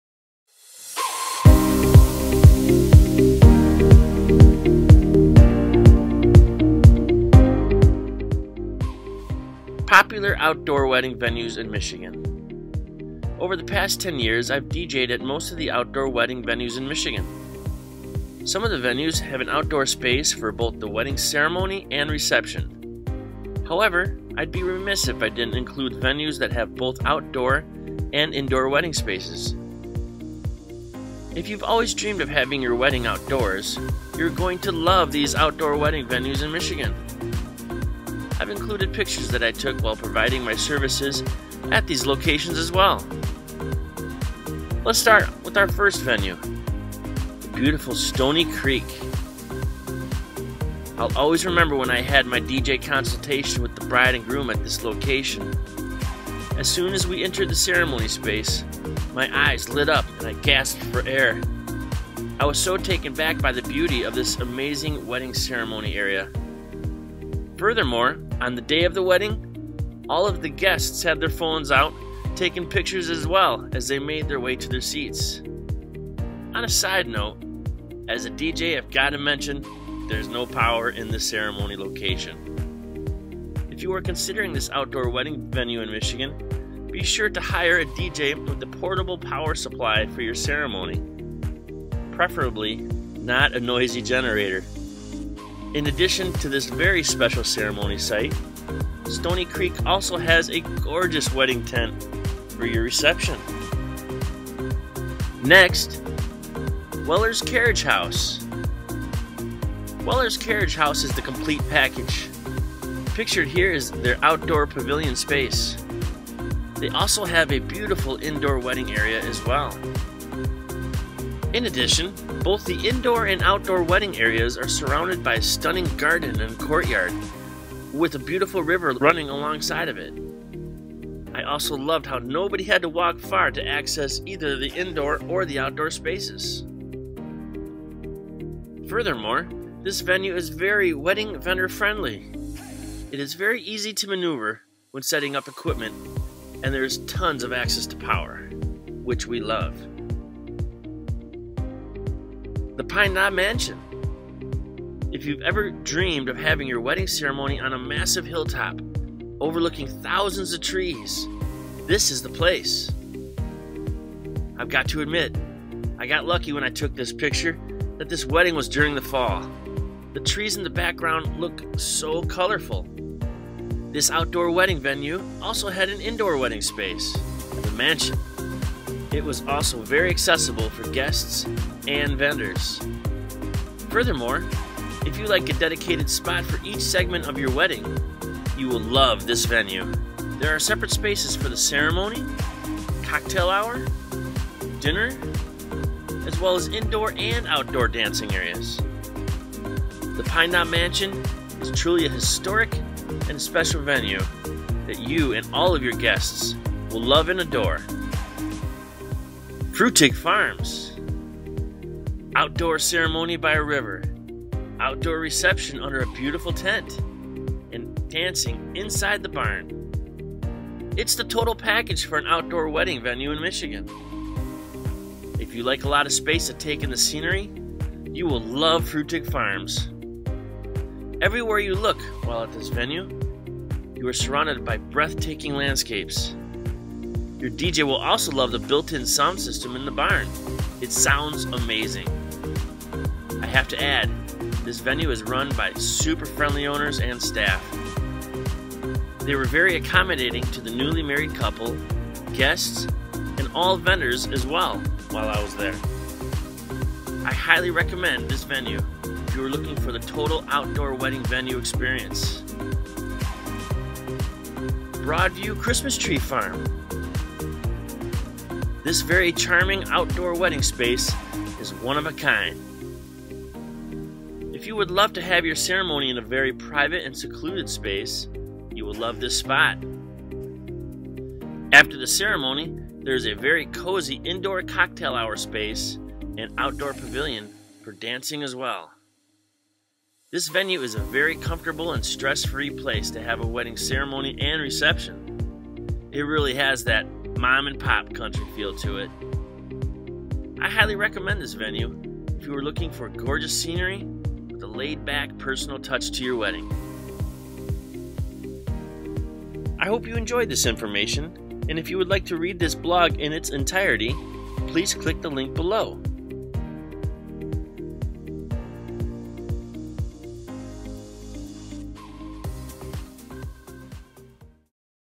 Outdoor Wedding Venues In Michigan
Check out thes amazing outdoor wedding venues in Michigan!

CC King Entertainment
Personalized Wedding Celebrations
DJs, Photo Booths, Party Rentals and More!